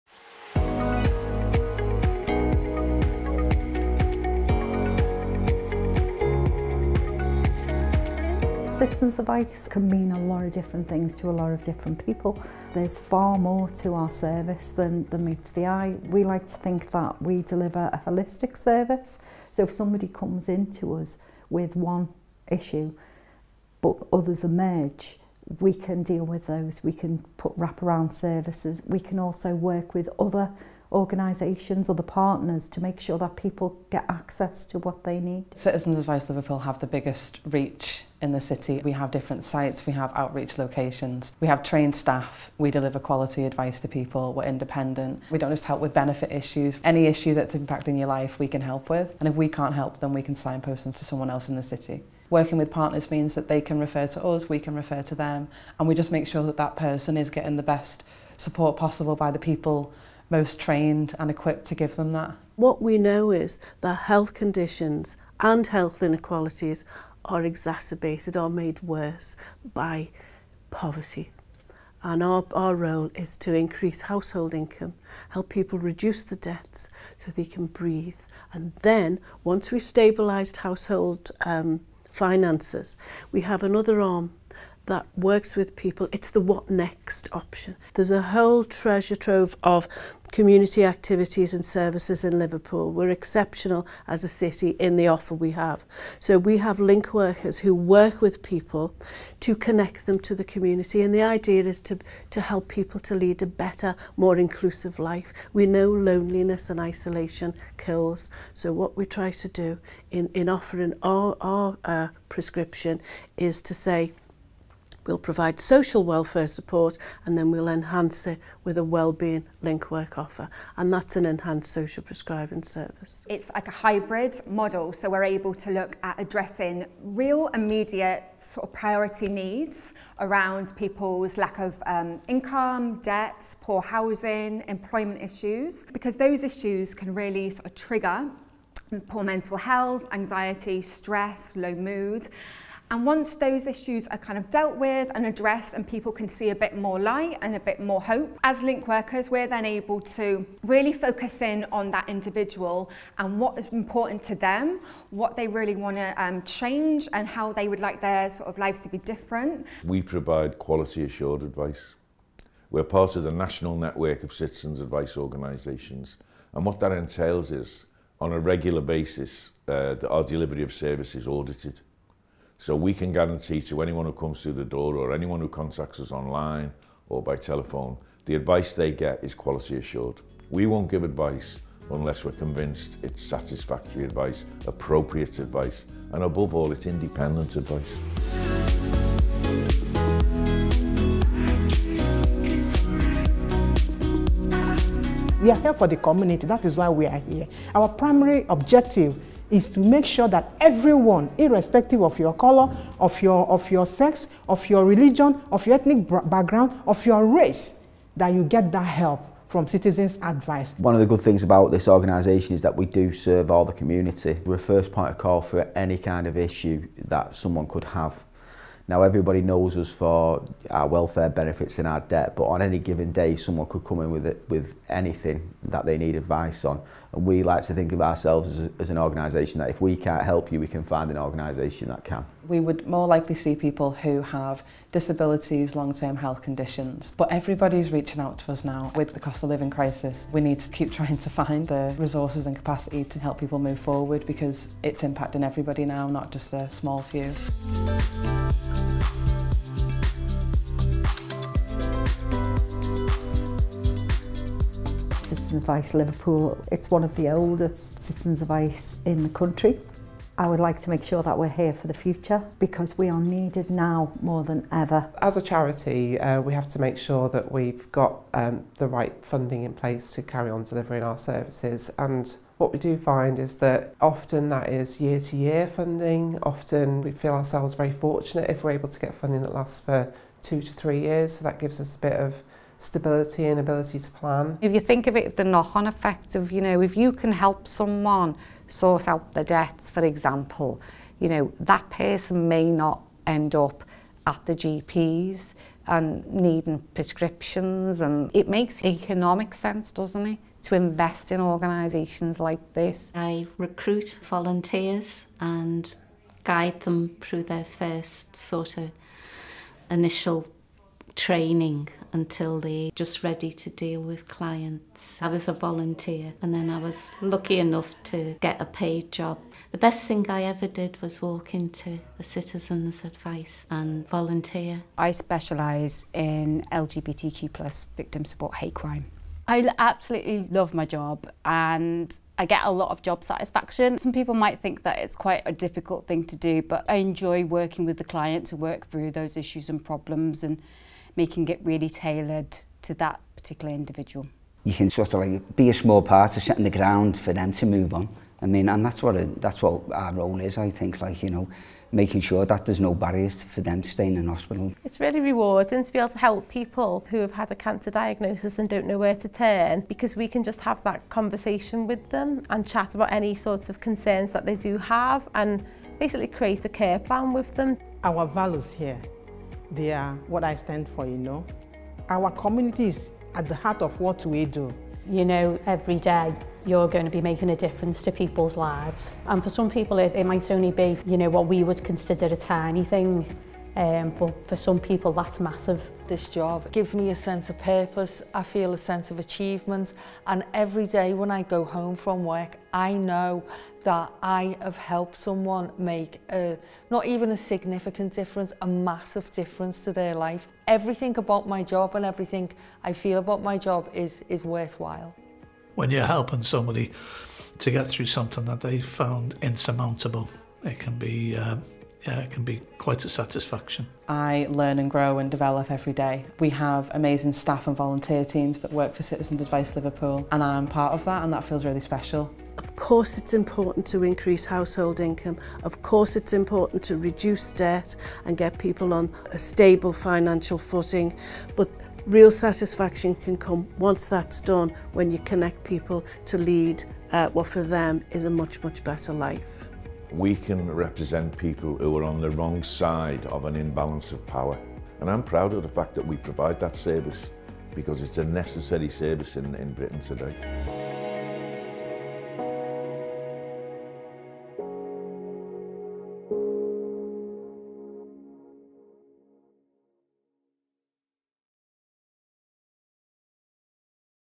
Citizen Advice Liverpool - Who We Are and What We Do
Citizens Advice Liverpool provides free, confidential and impartial advice to people who live and work in the city.

Our Mission: We give people the knowledge and the confidence they need to find their way forward – whoever they are, and whatever their problem.

We are the leading provider of advice and advocacy services in Liverpool.

Our Values: We value diversity, we promote equality and we challenge discrimination.